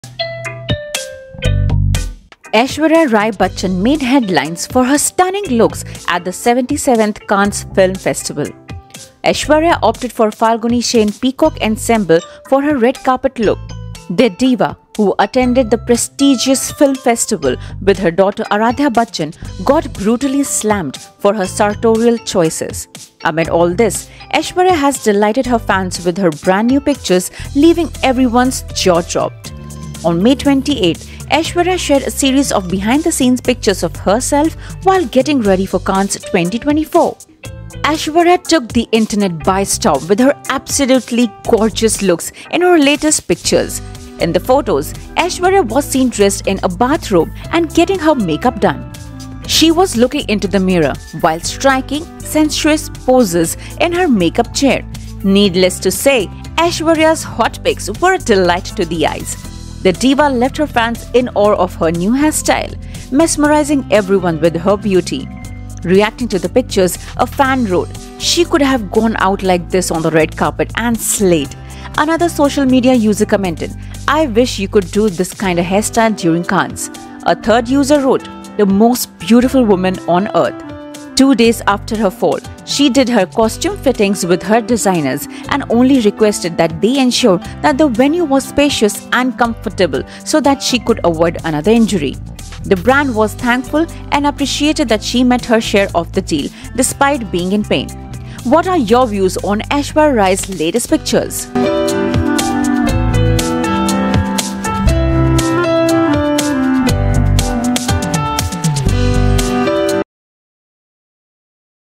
Aishwarya Rai Looks so Stunning after her new Transformation after Lip surgery and Botox
Aishwarya Rai Bachchan Looks so Stunning after her new Transformation with Daughter Aaradhya Bachchan after her Makeover